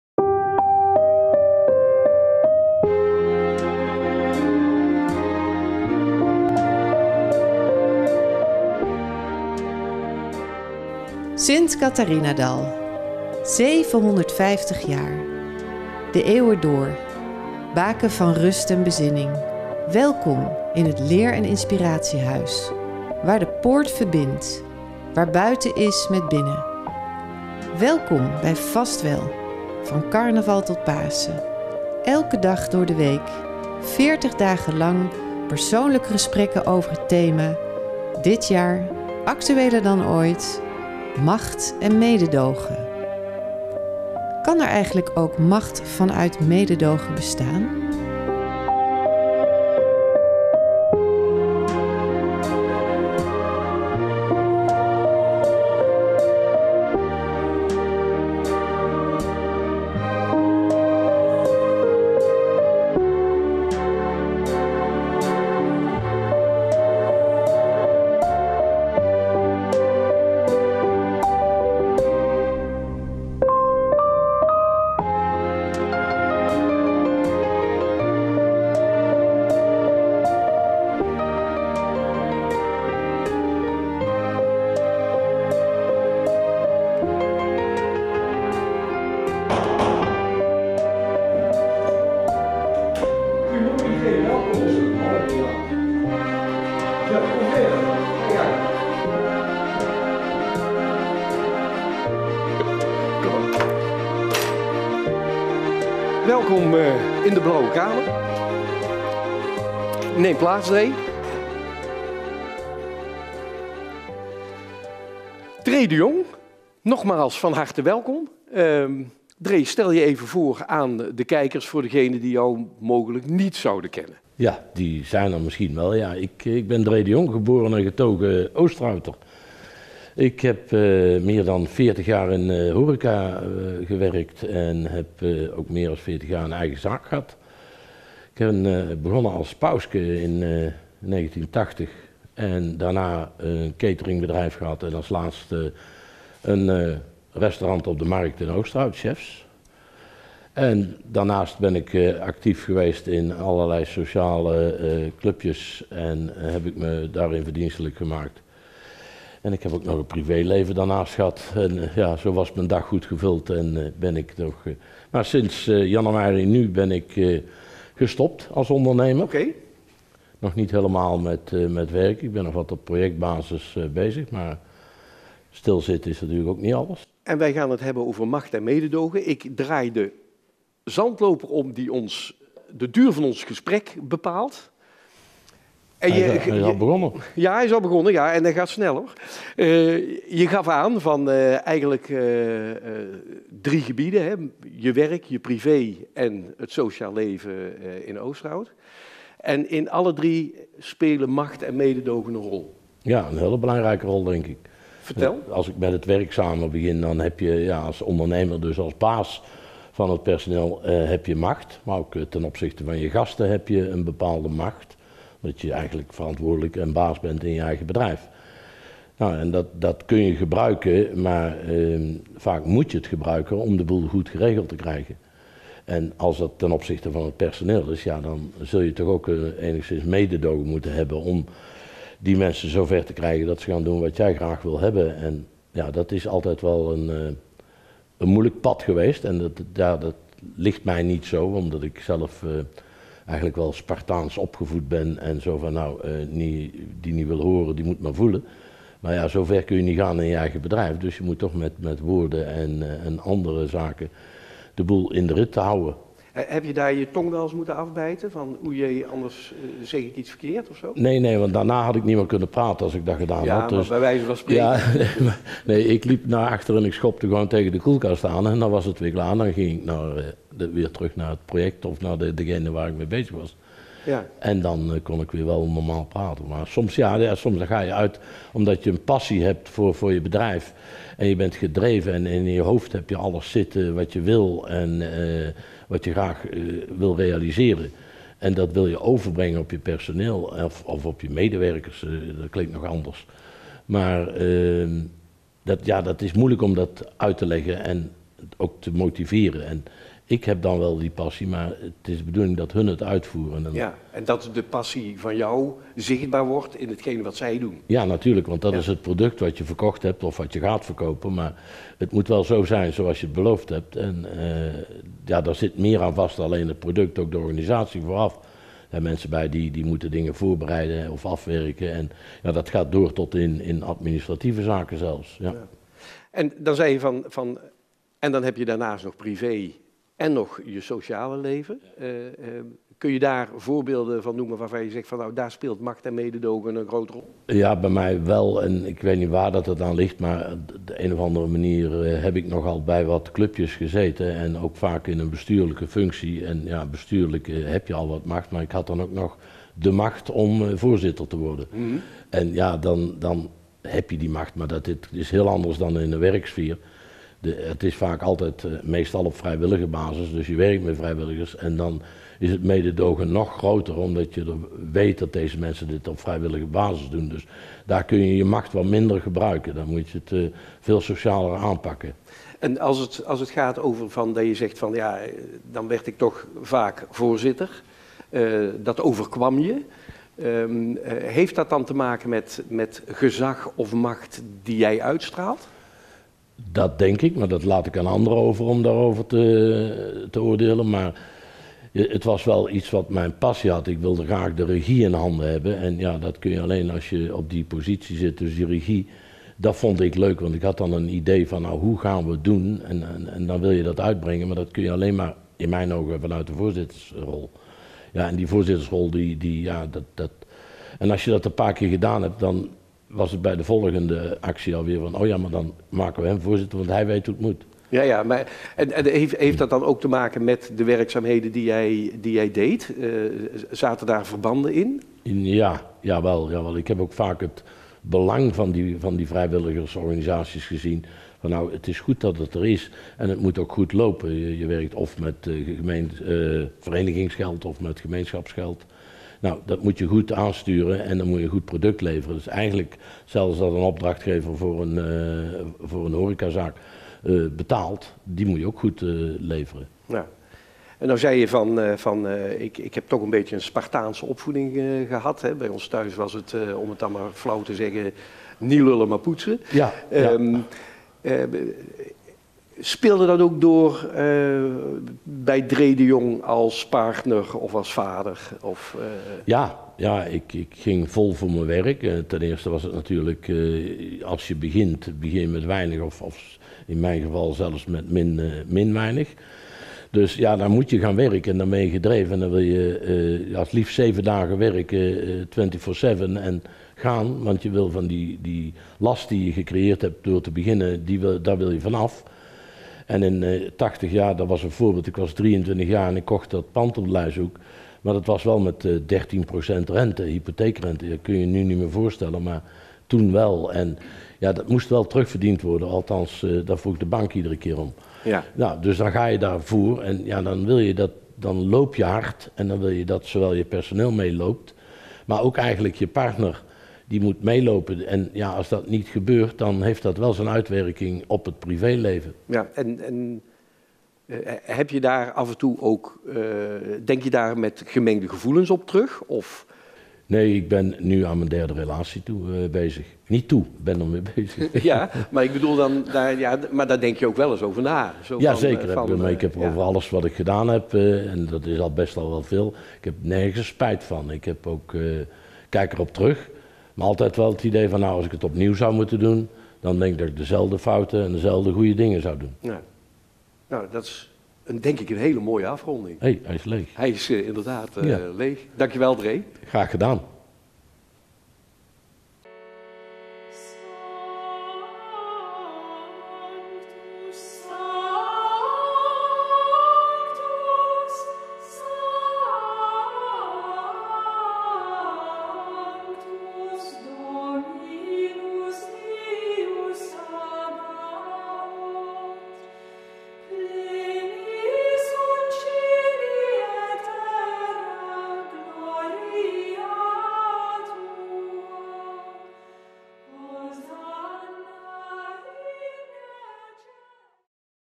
22 Vastwel Interview met Dré de Jong 26 mrt
22e interview in de serie Macht & Mededogen van Vastwel 2022. In deze aflevering Dré de Jong in de hoofdrol.
Wil je meer zien van de ORTS?
Volg onze social media kanalen!